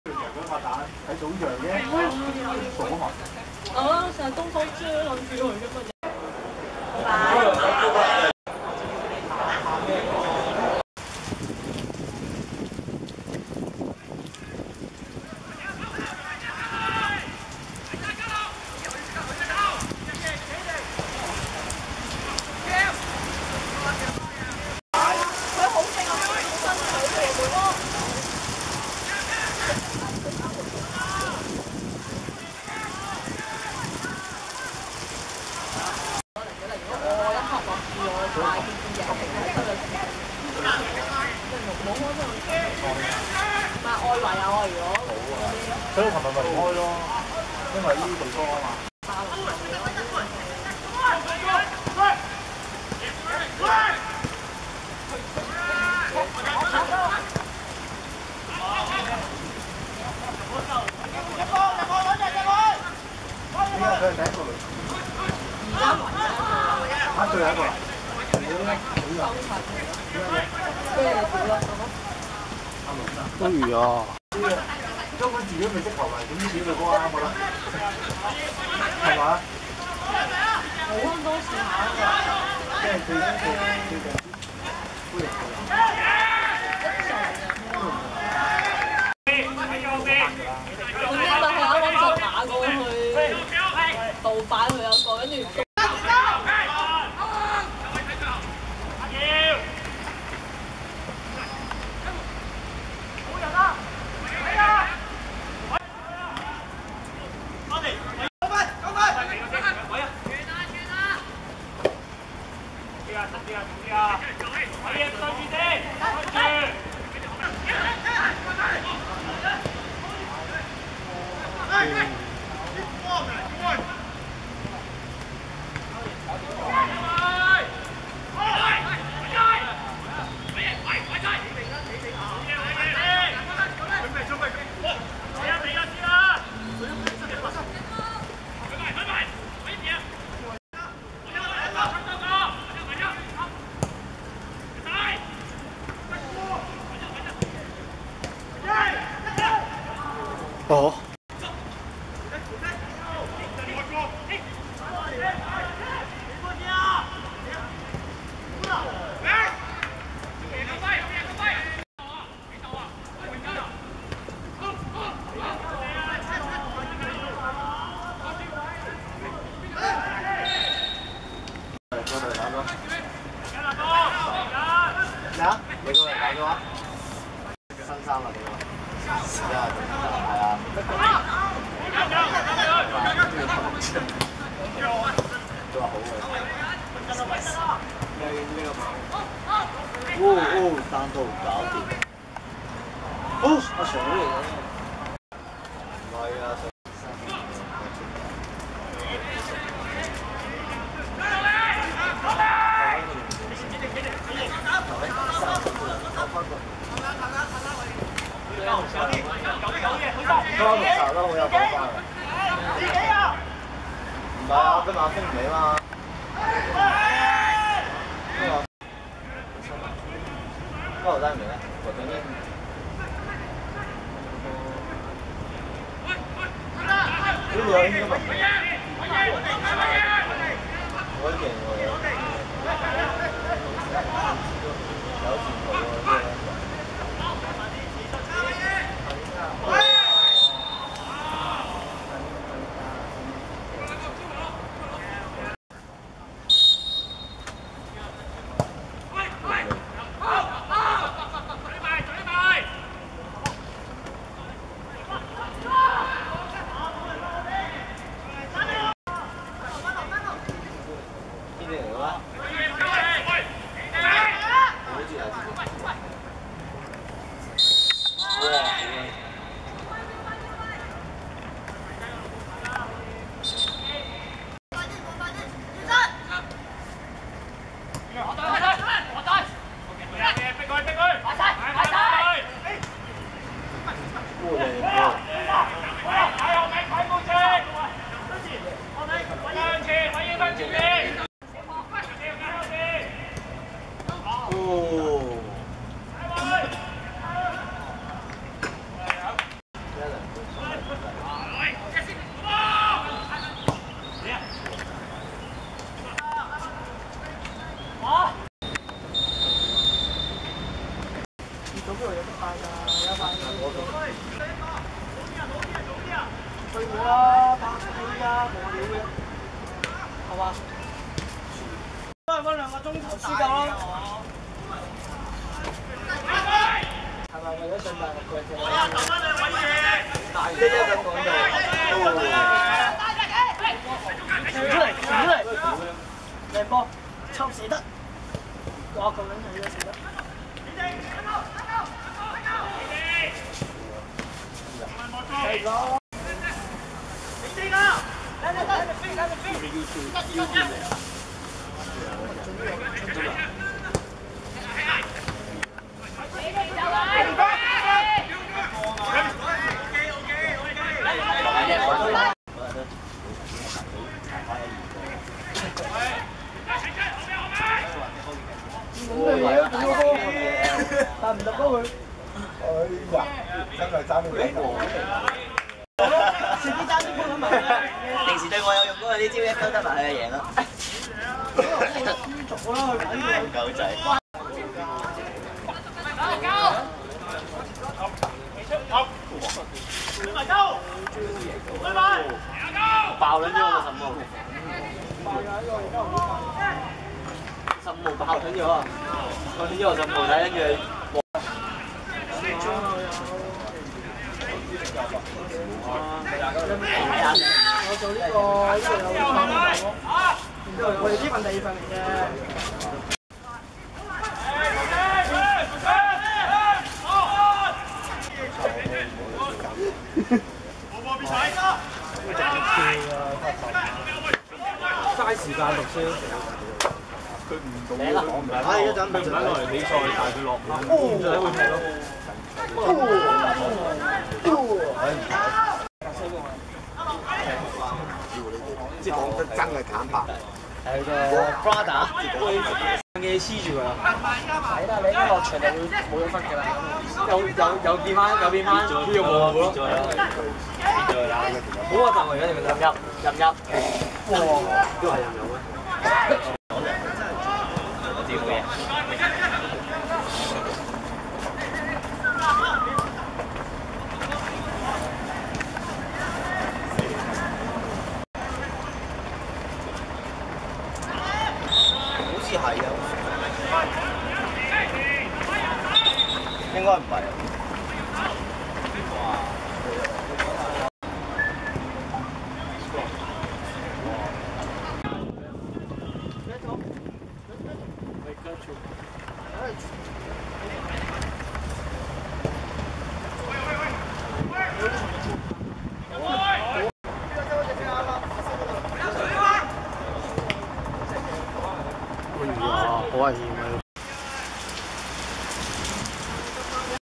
橫濱FC vs 太陽飛馬(2014.4.2.預備組足球聯賽)精華
2014.4.2.屯門湖山仿真草塲 18:30 橫濱FC(白) 2:2 太陽飛馬(黃)